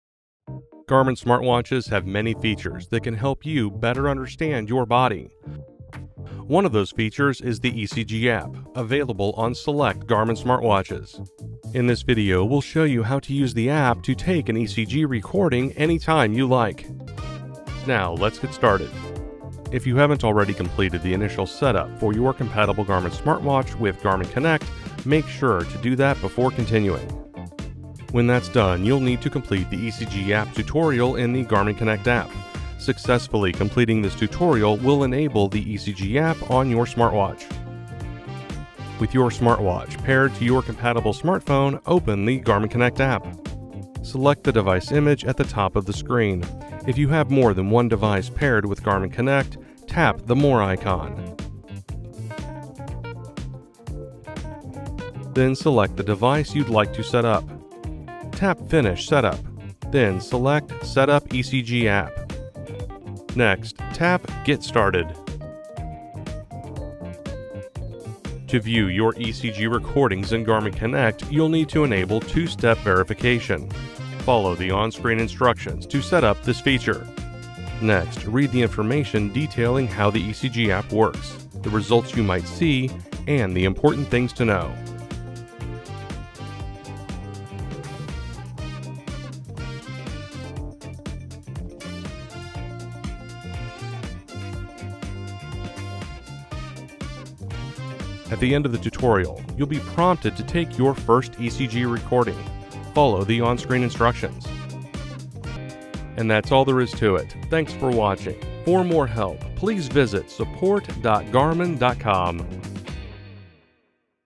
【教學】Garmin智慧手錶：ECG App | PART 2 | 在Garmin Connect上設定ECG App
本影片將告訴您如何在Garmin Connect App上啟用並設定ECG App功能。

0:00 影片介紹
0:17 在Garmin Connect上設定ECG App
1:45 結尾

*搭載最新版 Garmin Connect app 及手錶軟體的部分智慧型手錶。現在可下載使用心電圖應用程式＊ECG 應用程式並非在所有國家 / 地區都可使用，請前往 Garmin.com/ECG 了解適用情況。
＊ECG 應用程式非供 22 歲以下者使用。
＊搭配 ECG 應用程式使用，Garmin智慧手錶即可產生類似單通道心電圖的 ECG。